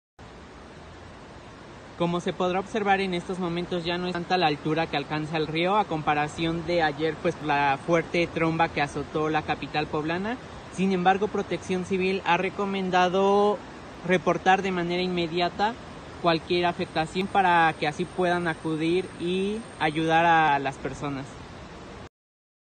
El río Alseseca luce con una altura normal, sin embargo Protección Civil debe estar al pendiente
En estos momentos el río luce con una altura normal, sin embargo si llegara a subir el nivel, se recomienda reportar de manera inmediata a Protección Civil Municipal